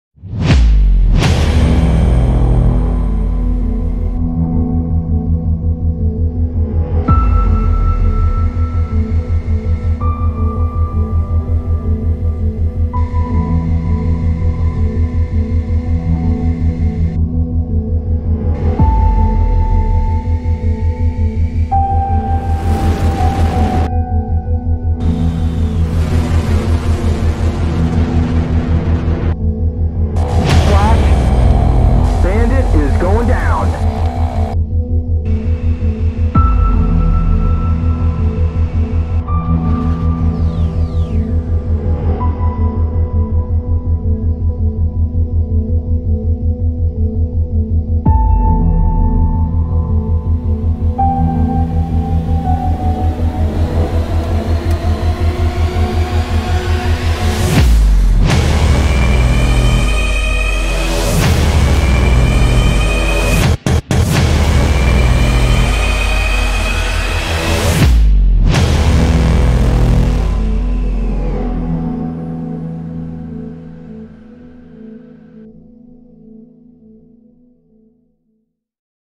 F 14AM Trailer
Hello, I made this video as a trailer for my squadron, this is not a mod for the F-14AM variant but a modified lua script to add the R-33 and R-73 missile to the aircraft.